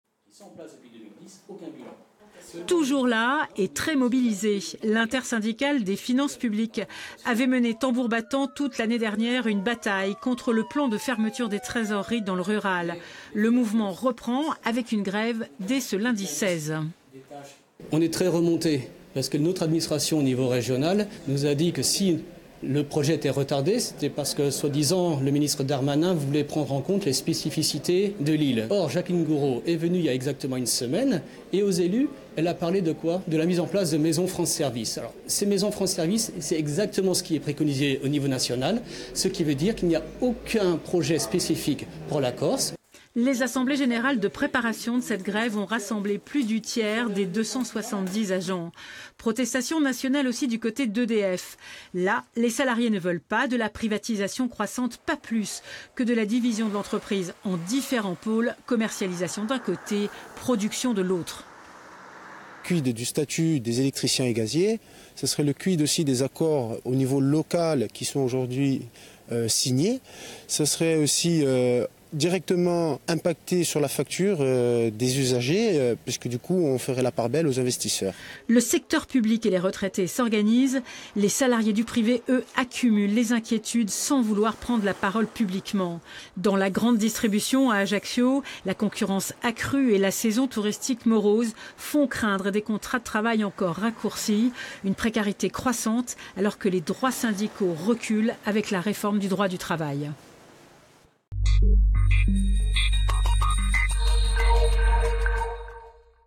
Corse : une rentrée sociale entre mobilisation et morosité
L'inquiétude se profile dans le privé face à une saison touristique moins bonne qu’à l’accoutumé. Dès le lundi 16 septembre, c'est dans la fonction publique que des conflits sont déjà prévus, avec un appel à la grève de l’intersyndicale des finances publiques.

Intervenants - Matthieu Caillaud, Secrétaire départemental FO Finances 2A ; Yannick Boutry, Secrétaire régionaladjoint CGT Energie.
Equipe - Florence Antomarchi ; Stéphane Agostini ; Fabien Bernardini.